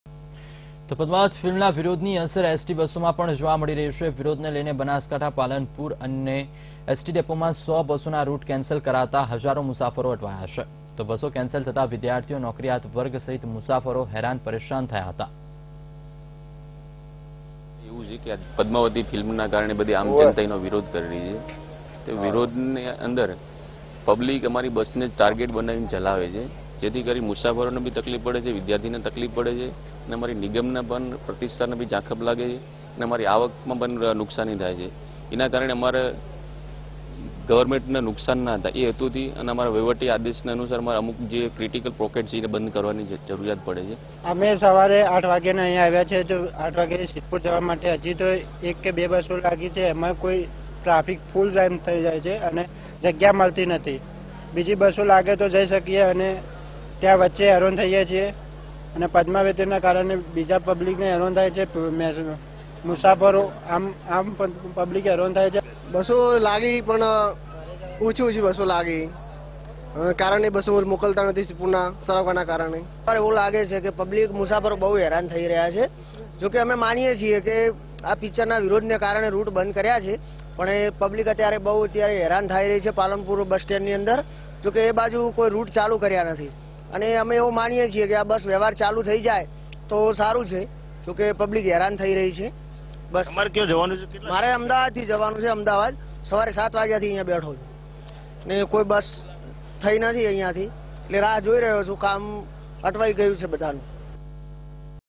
Protest of Padmavati halted 100+ ST Bus Service in North Gujarat; Daily Commuters upset | Vtv News
Protest of Padmavati halted 100+ ST Bus Service in North Gujarat; Daily Commuters upset